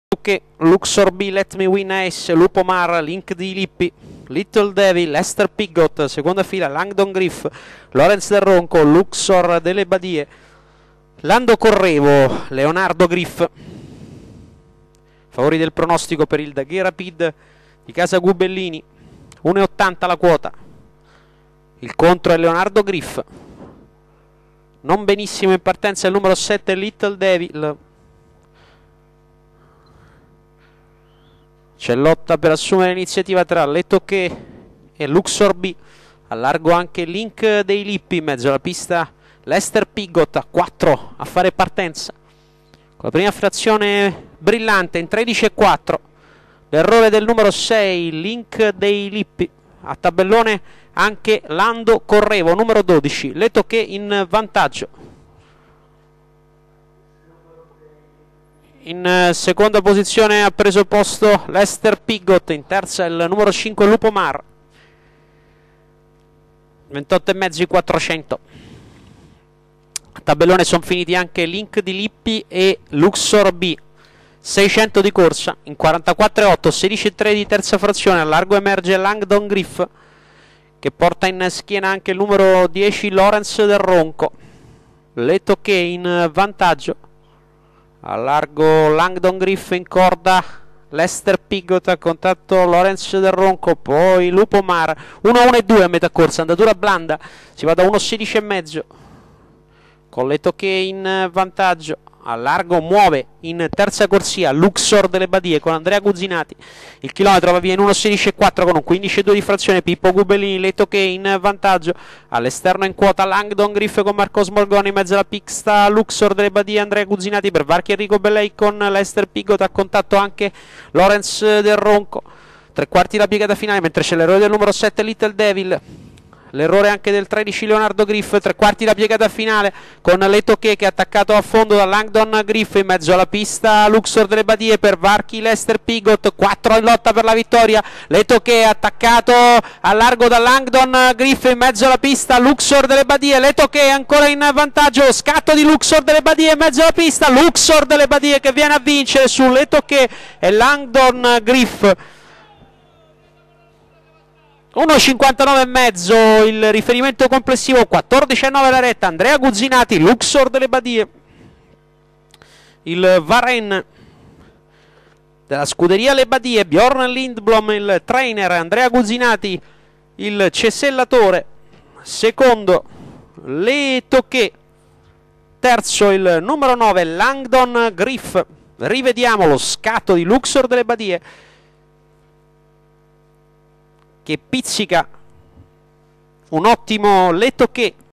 Berardelli 2008 - LUXOR DELLE BADIE
Luxor delle Badie (Varenne & Babilonia Dei) vince in 1.14.7 su Le Touquet e LANGDON GRIF